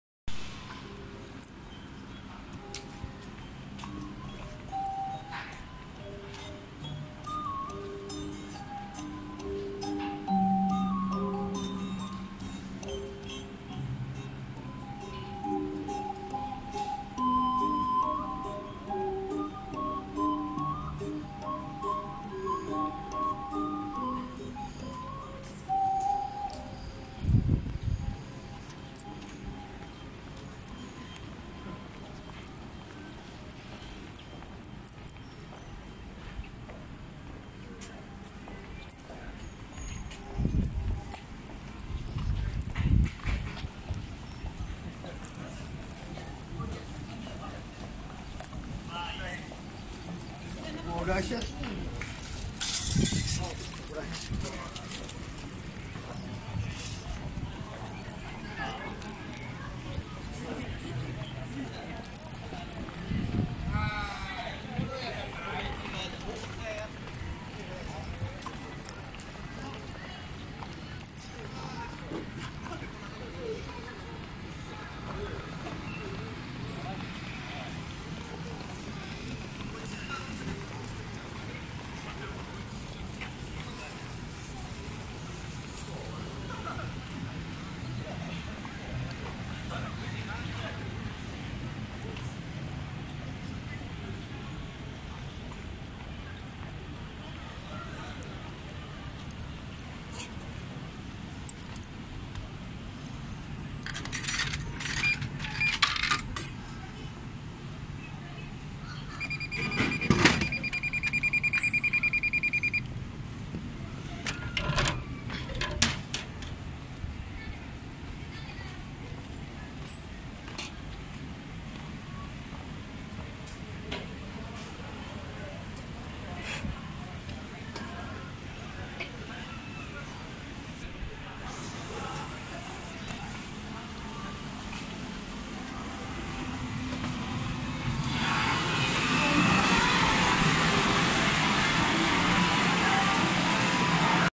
新小岩北口 サミット前裏路地、駅前、ぼったくり店 20190606 21時22分
癒し美容室のBGMが流れていた♪